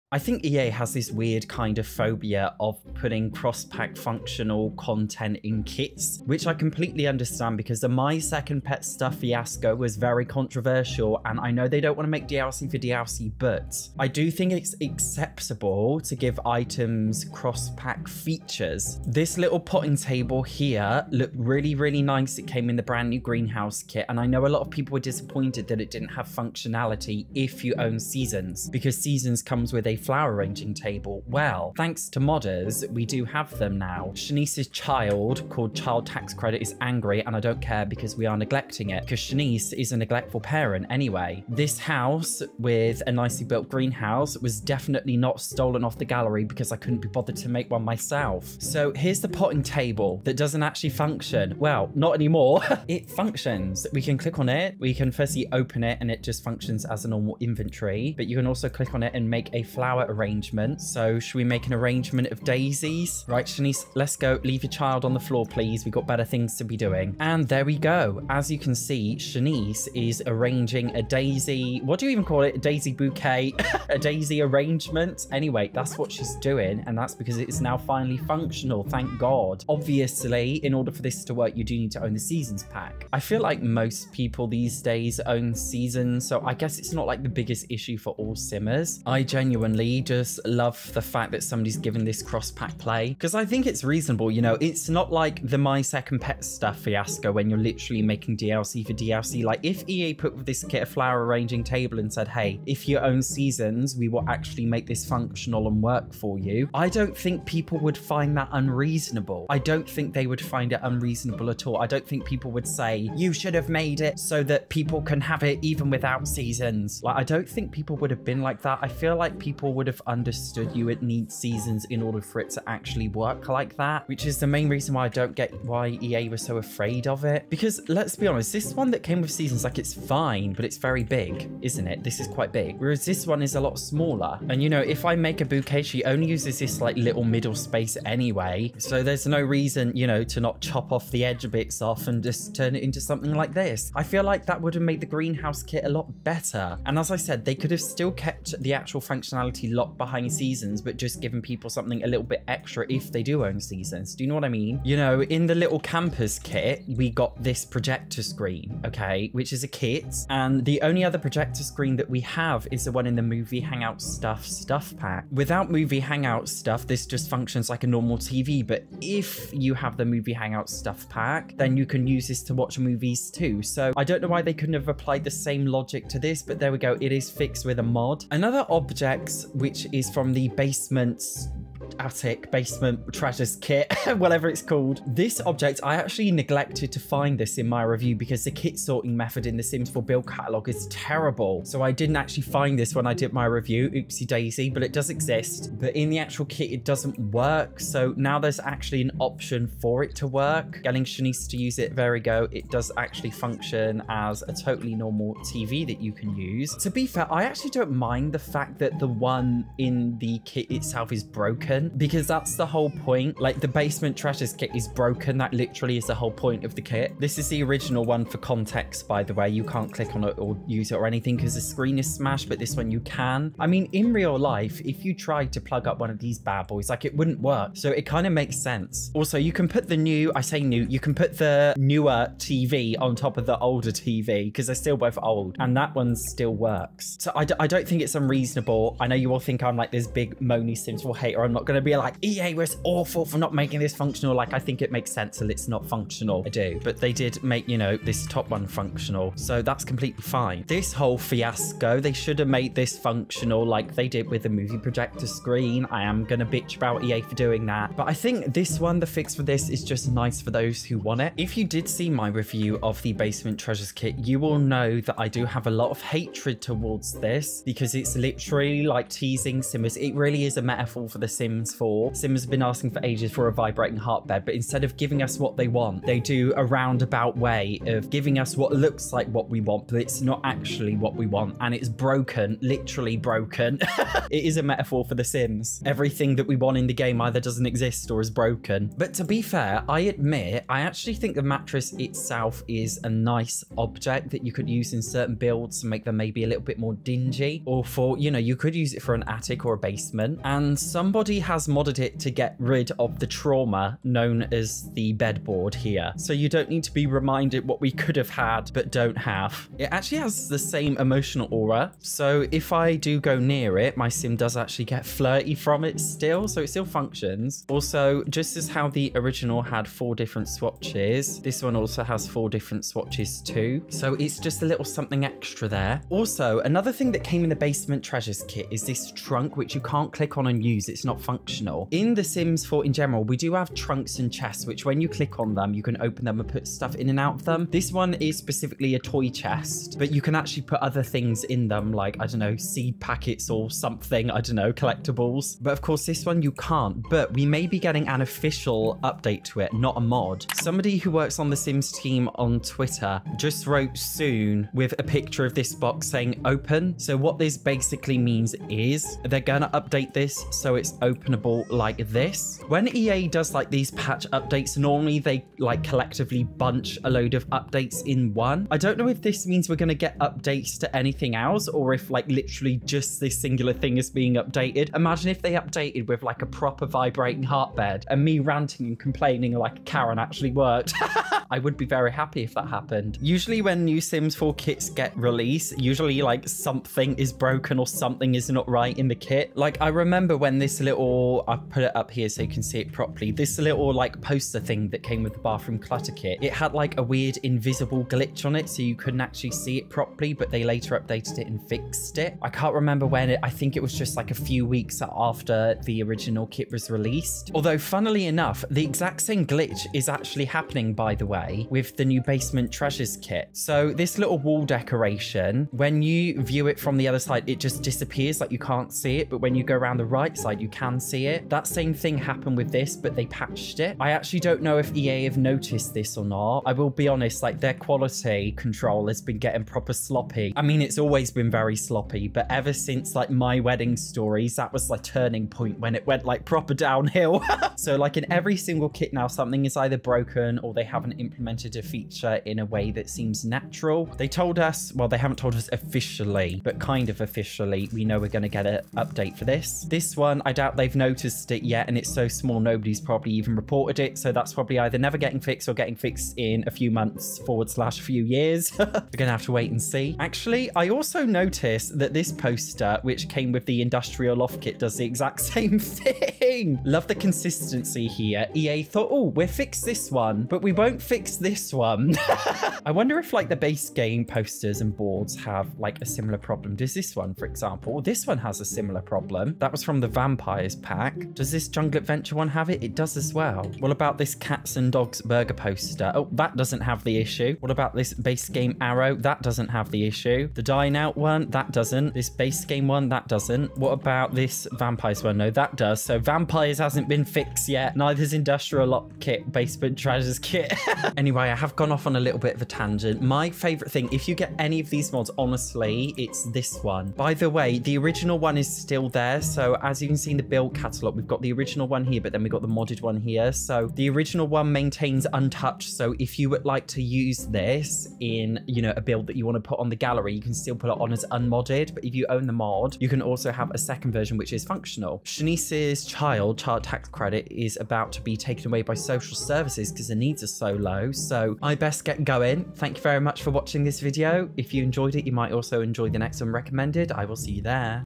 The new Sims 4 Kits have been fixed by modders! (Greenhouse Haven Kit/Basement Treasures Kit)
The Sims 4 Basement Treasures Kit and The Sims 4 Greenhouse Haven Kit have been fixed by modders! This is a Sims 4 kit videos discussing the new sims 4 kits released in April 2023.

*im not wearing lipstick my lips just naturally look like this.

If you enjoyed the video please hit like as it supports me more than you think and helps me to grow! :)